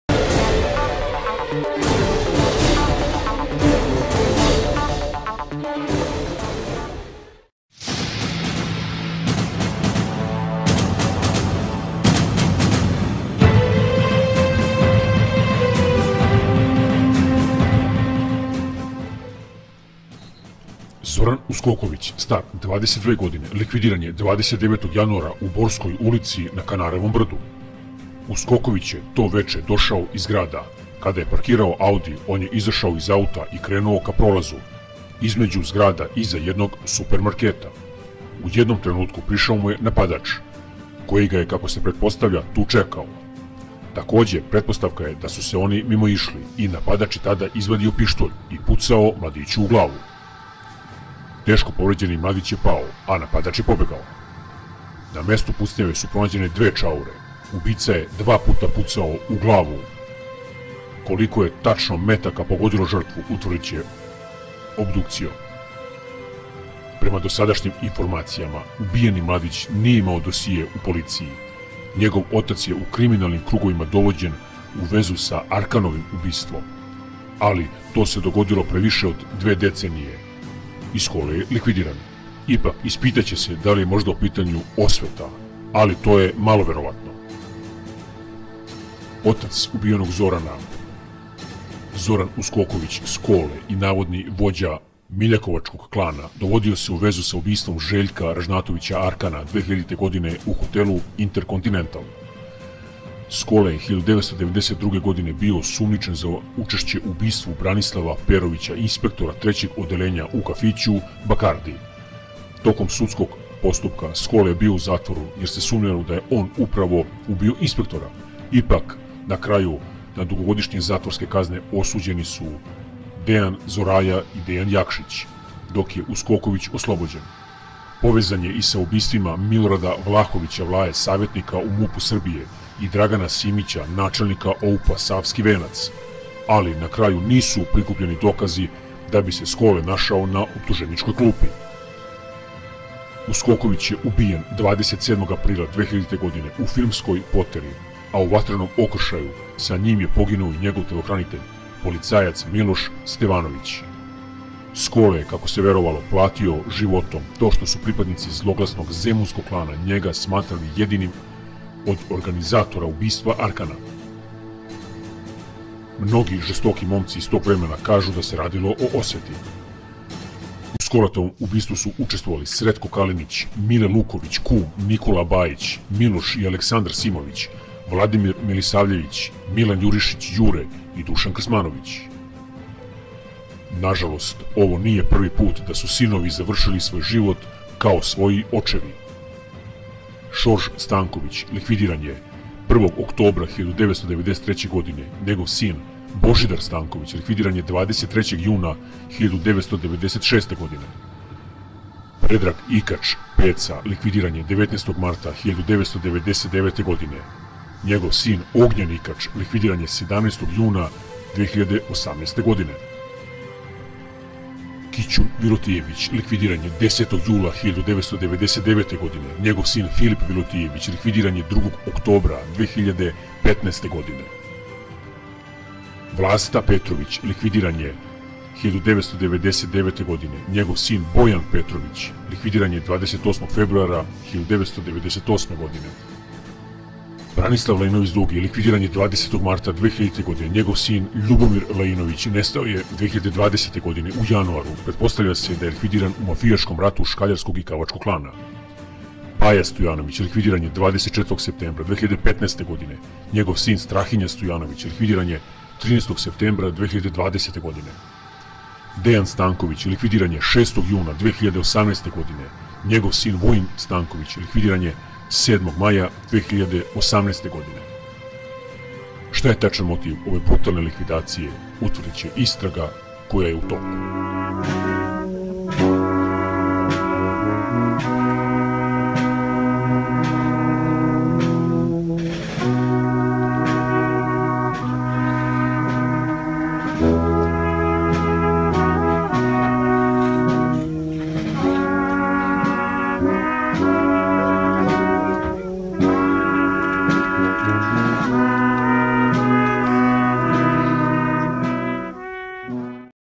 ZORAN USKOKOVIĆ(22) 29.01.2021
MASKIRAO SE U STARCA PA IZREŠETAO ZORANA USKOKOVIĆA: Oslanjao se na štap, a onda je izvadio pištolj!
Ubistvo Zorana Uskokovića (22) zabeležile su kamere za video-nadzor koje su snimile sam čin likvidacije, koje je izveo atentator maskiran u starca. Čovek je bio maskiran, nosio je šubaru na glavi i bio je obučen u crni mantil, a pomagao se štapom dok je hodao. Mladi Uskoković nije ni slutio šta mu se sprema.
Stravično mafijaško ubistvo se dogodilo u petak, pola sata pre ponoći, pored prodavnice u beogradskom naselju Kanarevo brdo u Borskoj ulici.
Prema nezvaničnim saznanjima, Skoletov sin je parkirao sivi "audi" karavan na parkiralištu prodavnice i krenuo kroz prolaz prema zgradi u kojoj mu žive majka i očuh. Kako se vidi na snimku nadzorne kamere, Zoran je hodao i gledao u telefon, a u susret mu je polako išao muškarac, koji je na prvi pogled izgledao kao starac.
- Ubica je na glavi imao šubaru i crni mantil, a u levoj ruci štap, kojim se pridržavao dok ide. Kad je prišao Uskokoviću, iznenada je iz džepa izvadio pištolj s prigušivačem i pucao mladiću u lice. Zoran je istog trenutka pao na leđa, a ubica mu je prišao još bliže i iz neposredne blizine ga "overio" ispalivši u njega još nekoliko hitaca, nakon čega se mirno udaljio - kazao je naš izvor. On dodaje da se na snimku ne vidi lice ubice jer je nosio hiruršku masku. - Zoranovo telo su tek narednog jutra pronašli radnici prodavnice kad su došli na posao. Tokom uviđaja, na mestu zločina pronađene su četiri čaure, koje su poslate na veštačenje - ispričao je naš sagovornik.
Prema njegovim rečima, o motivima ove mafijaške sačekuše za sada može samo da se nagađa.
- Skoletov sin nije imao kriminalni dosije u policiji i kažnjavan je samo zbog nekih sitnih prekršaja. U kriminalnim krugovima je nepoznat, ali će inspektori svakako proveriti sve njegove kontakte, kako bi se utvrdilo da li se bavio nelegalnim radnjama, i da li se nekom zamerio u podzemlju, pošto je za njegovo ubistvo angažovan profesionalni ubica. Druga varijanta, iako manje verovatna, je da iza svega stoji krvna osveta za ubistvo Željka Ražnatovića Arkana iz januara 2000. godine, za šta se sumnjičio Zoranov otac Zoran Uskoković Skole.
Podsetimo, Skole je likvidiran u spektakularnoj jurnjavi automobilima na Vidikovcu 27. aprila 2000, samo tri meseca nakon što je ubijen Arkan u tadašnjem hotelu "Interkontinental". U kriminalnim krugovima tada se pričalo da je Skole organizator Arkanovog ubistva, ali on se pravdao da nema veze sa zločinom u "Interkontinentalu", da je bio prijatelj sa Ražnatovićem, da je udovici Svetlani Ceci Ražnatović izjavio saučešće i dao čitulju u novinama. Međutim, "zemunski klan" je odmah posle Arkanovog ubistva krenuo u osvetu, pa je ubrzo na red došao i Skole.
Tog dana, 27. aprila 2000. oko 16 časova, Uskoković je bio u "audiju" sa vozačem Petrom Jokićem i telohraniteljem, policajcem Milošem Stevanovićem, kad je na uglu ulica Kneza Višeslava i Pilota Mihajla Petrovića u naselju Cerak vinogradi, na njih otvorena vatra iz jednog automobila.
Na rafale iz automatskog oružja Skole i Stevanović su uzvratili hicima iz pištolja i počela je žestoka jurnjava i pucnjava po Ceraku i Vidikovcu, što je trajalo nekoliko minuta. Ubice su na kraju sustigle Skoletov "audi" i likvidirale njega i telohranitelja, dok je vozač Jokić samo lakše ranjen.
Sretko Kalinić, zvani Zver, jedan od najkrvoločnijih egzekutora "zemunskog klana", ispričao je 15 godina kasnije u sudu: - Moj recept za likvidacije beogradskog ološa traje pet do šest sekundi. Najduža likvidacija trajala mi je minut i po, a reč je o ubistvu Zorana Uskokovića Skoleta.
Inače, Skole je devedesetih godina važio za "gospodara" Rakovice. Bio je u žiži javnosti 1992, kada je uhapšen pod sumnjom da je umešan u ubistvo Branislava Perovića, inspektora Odeljenja za rasvetljavanje ubistava beogradske policije. Perović je ubijen u kafiću "Bakardi" na Banjici, prilikom legitimisanja gostiju. Skole je nakon suđenja oslobođen za ovo delo, ali sumnjičio se za ubistvo još dva policijska inspektora, za šta takođe nikad nije odgovarao.
UBICA N.N